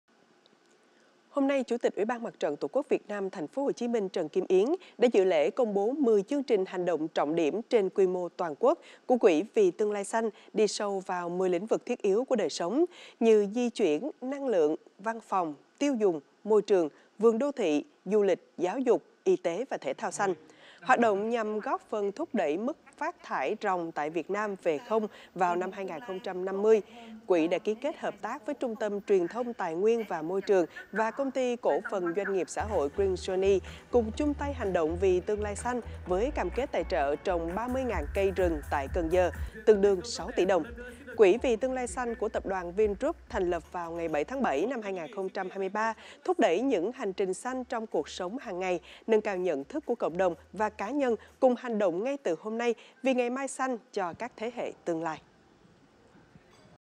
QUỸ "VÌ TƯƠNG LAI XANH" CÔNG BỐ 10 CHƯƠNG TRÌNH HÀNH ĐỘNG TRỌNG ĐIỂM
Quỹ "Vì Tương lai xanh" của tập đoàn Vin group thành lập vào ngày 7/7/2023 thúc đẩy những hành trình xanh trong cuộc sống hàng ngày, nâng cao nhận thức của cộng đồng và cá nhân cùng hành động ngay từ hôm nay vì ngày mai xanh cho các thế hệ tương lai.